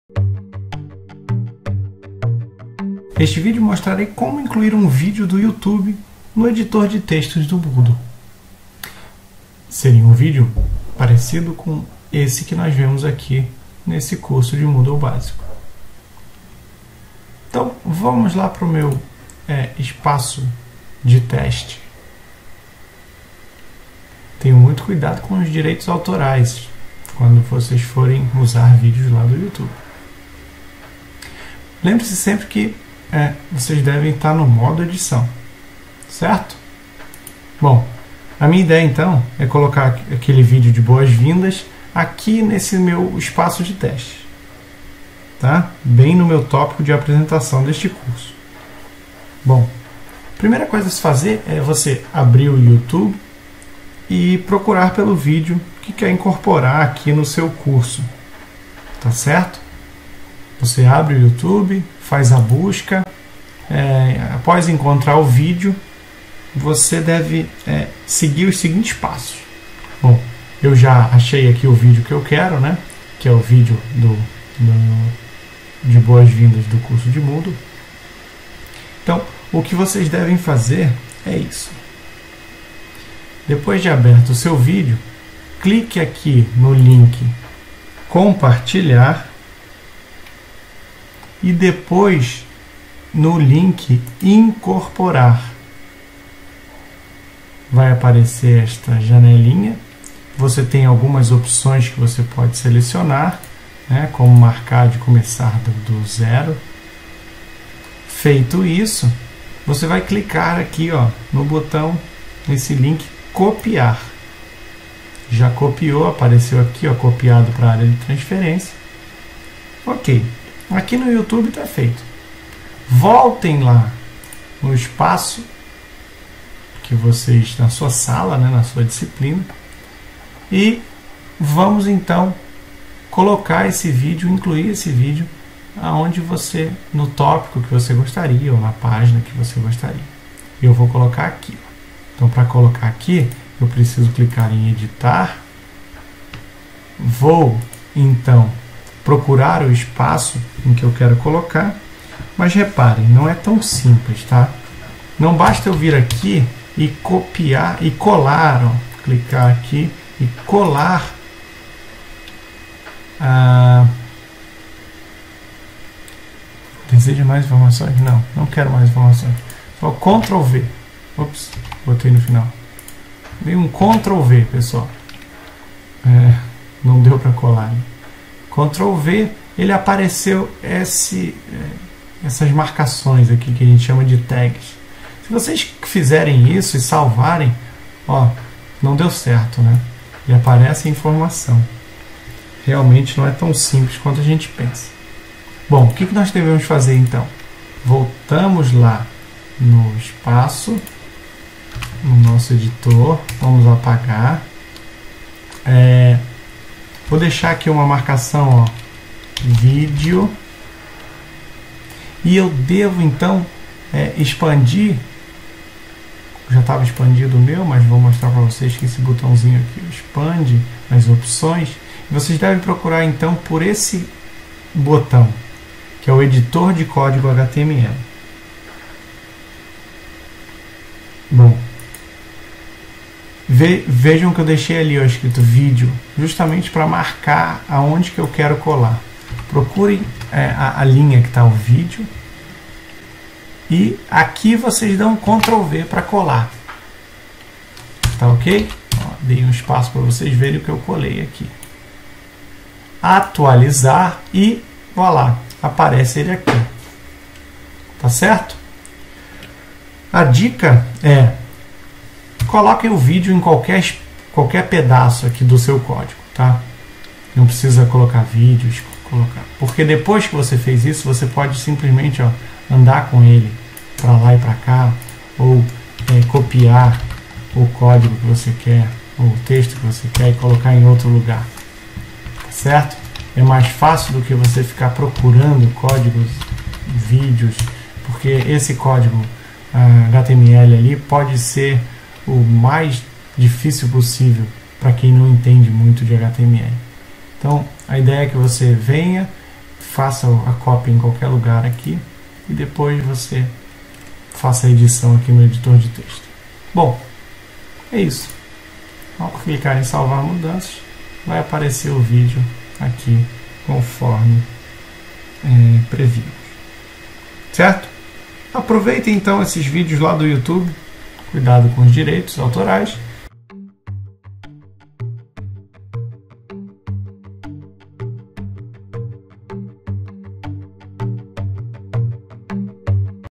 MOODLE BÁSICO - Recurso: Inserir um Vídeo do Youtube
É possível incorporar um vídeo do Youtube no ambiente Moodle? Claro que sim!
Como você pode fazer uso de mais esse recurso e assim tornar o ambiente da sua disciplina mais interativo? Ao assistir o tutorial você verá que é mais fácil do que imaginava.